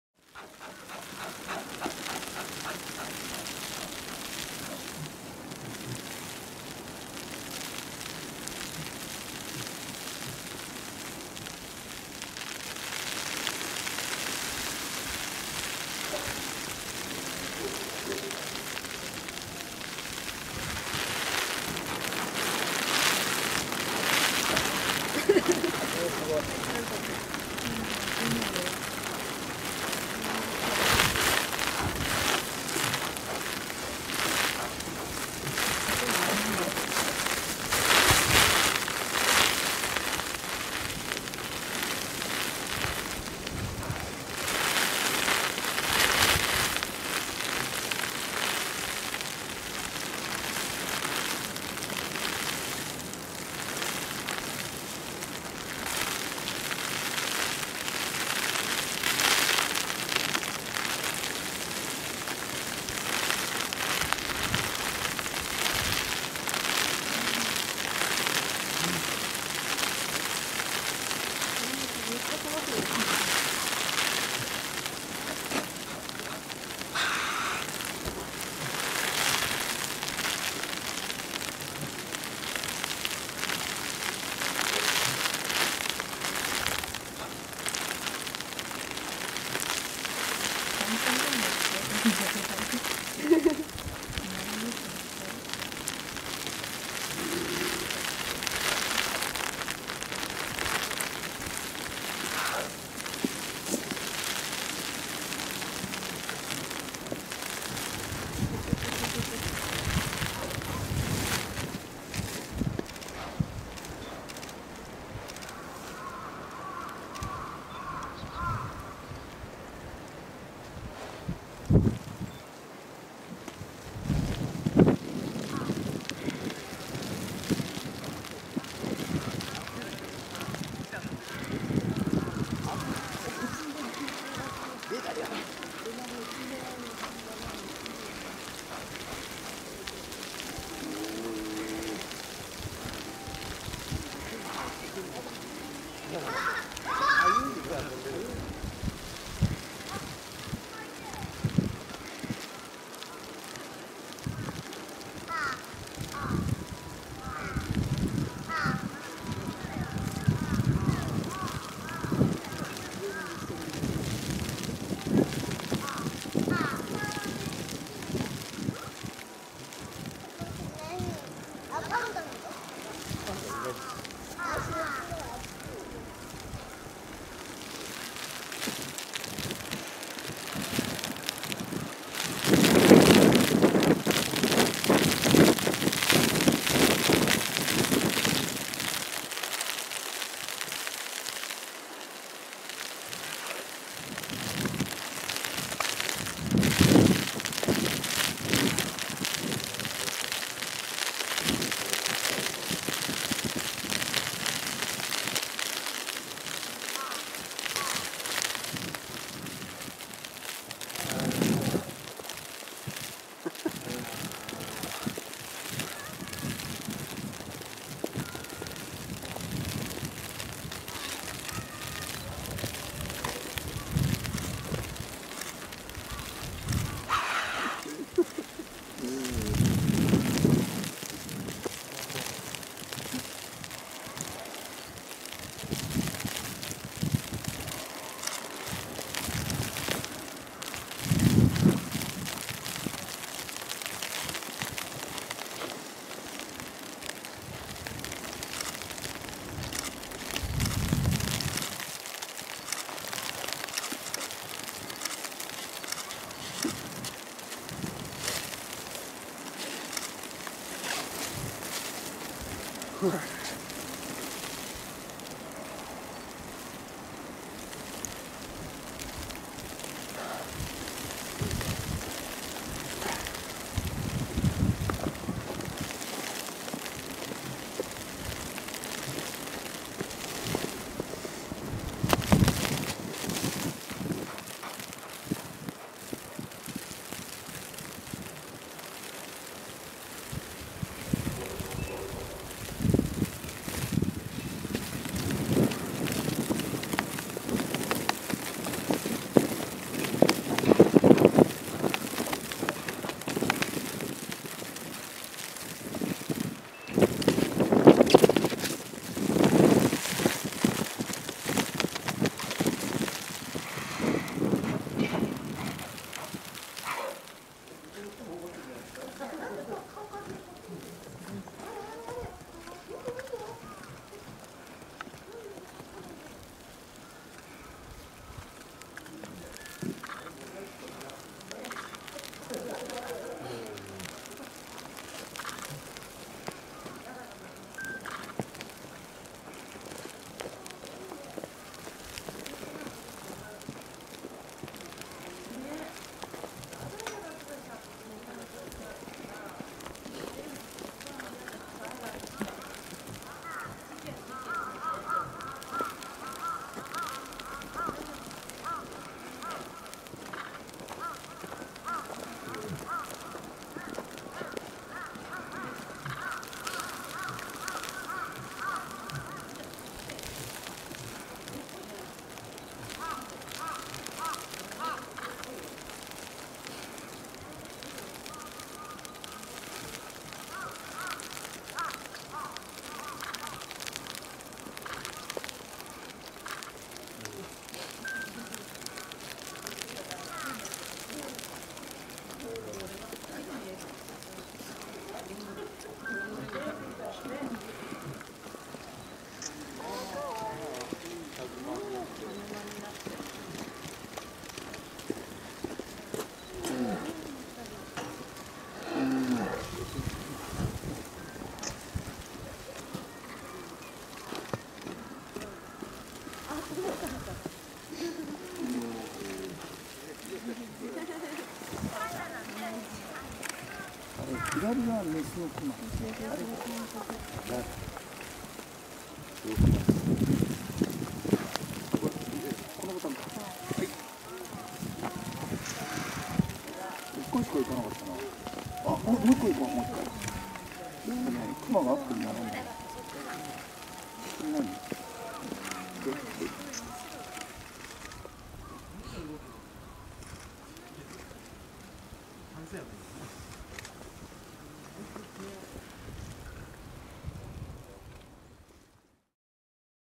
ホッキョクグマ デナリとララの同居開始~Polar Bear,The season of Love
円山動物園のホッキョクグマ、デナリ(オス/18歳)、ララ（メス/17歳)　。
繁殖のための同居が、17日(土)の閉園後から開始されました。
18日(日)の2頭の様子。
ララは、デナリに向かって、お尻を向けて立ち止まったりしています。
デナリは、ララが寝れば一緒に寝る、歩けば、一緒に歩き出す・・・と行動を共にし、
あまり離れません。
(2012年3月18日撮影)

[Denali] (male,18 years old)　[Lara] (female, 17 years old).
Living together is started for breeding.
(recorded on March 18, 2012)
URL: